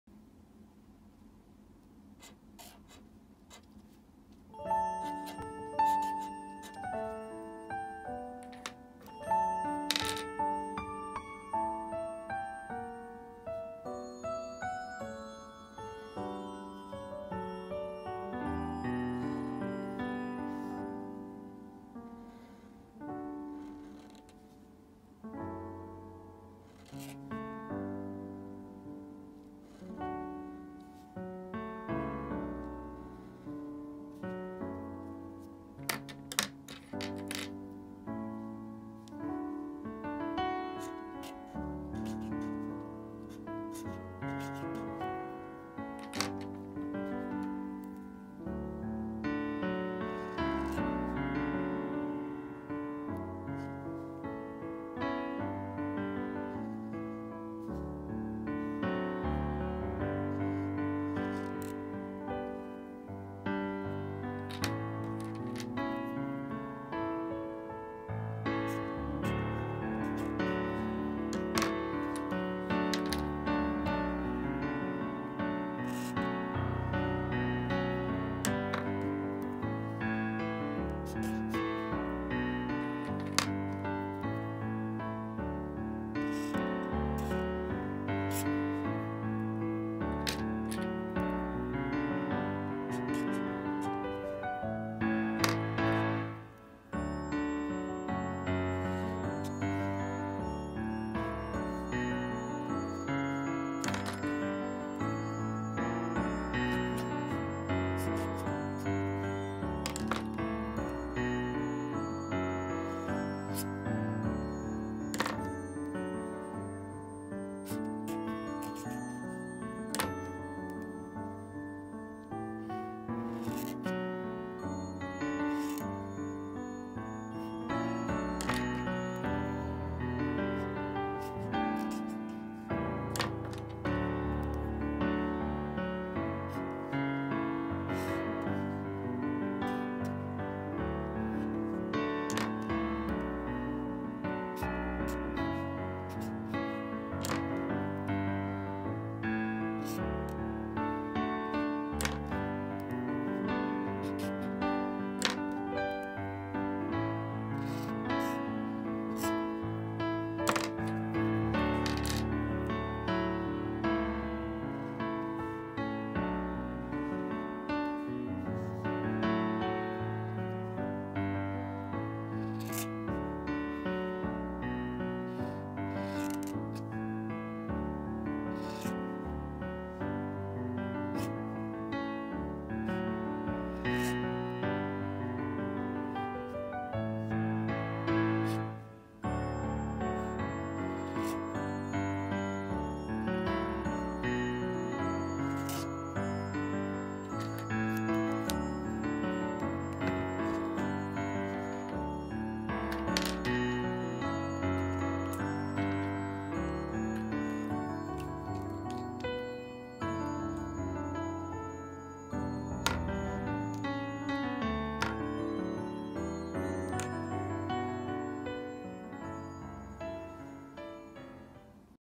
How to write “Nishikata kun" in japanese? “Teasing Master Takagi-san”(kanji,hiragana,katakana)
Let’s practice together!!

Enjoy study japanese!!

The order of writing is important for Japanese beautifully.

※If you have any requests please let me know!!

・Purple color = kanji
・Blue or Green color = hihagana
・Pink color = katakana
・Brown color = pronunciation